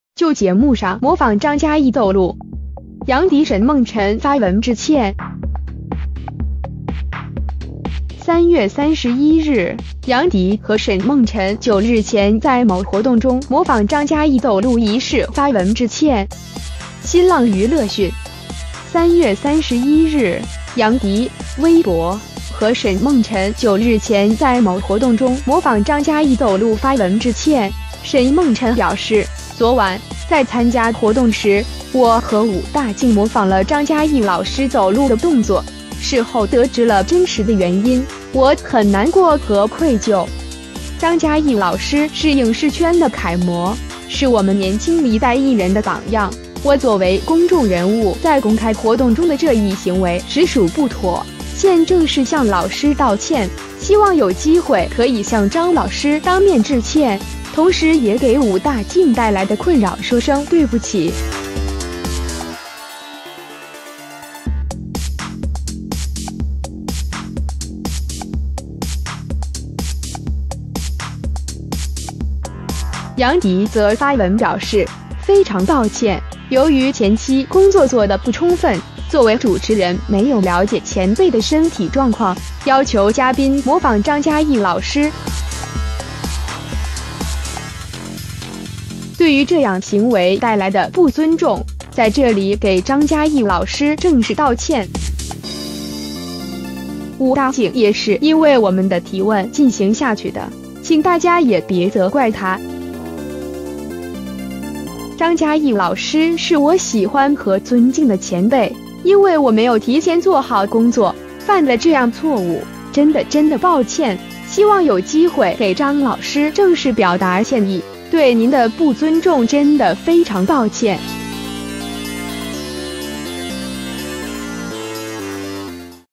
就节目上模仿张嘉译走路 杨迪沈梦辰发文致歉
3月31日，杨迪和沈梦辰就日前在某活动中模仿张嘉译走路一事发文致歉。